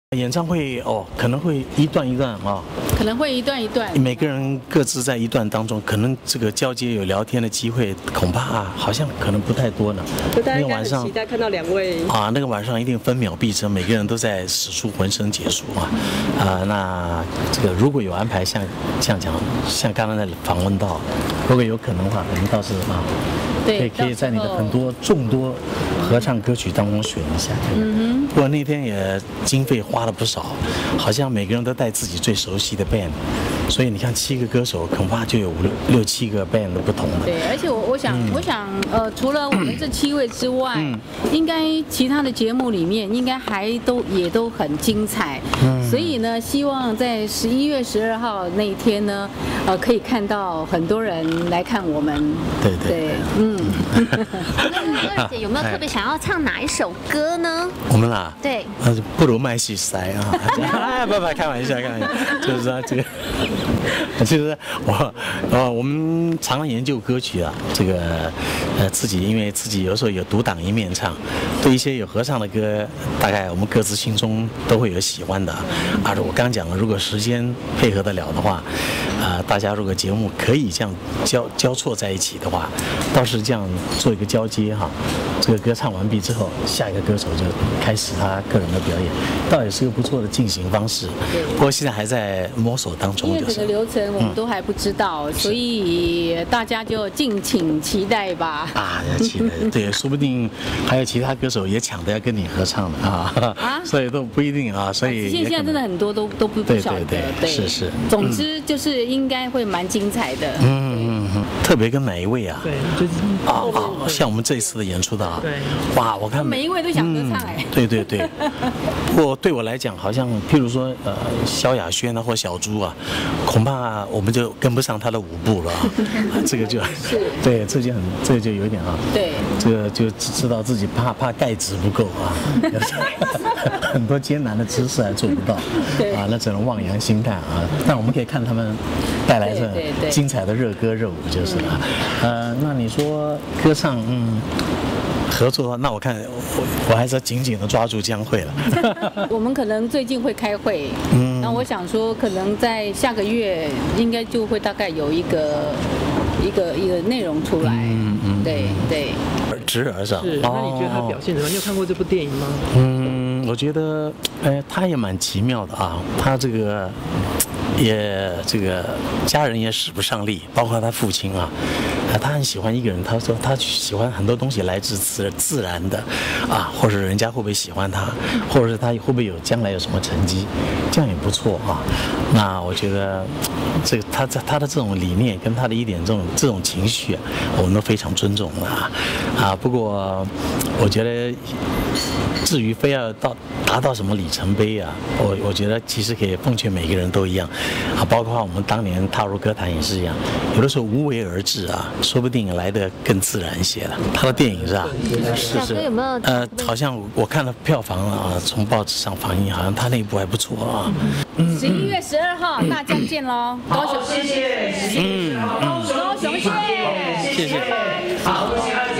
史上最強卡司 「聽見未來 夢想一百」
「聽見未來　夢想一百」演唱會，集合了史上最強卡司：江蕙、張惠妹、費玉清、瘐澄慶、蕭亞軒、羅志祥、蘇打綠。現場找來小哥費玉清與二姊江蕙，搞笑對話。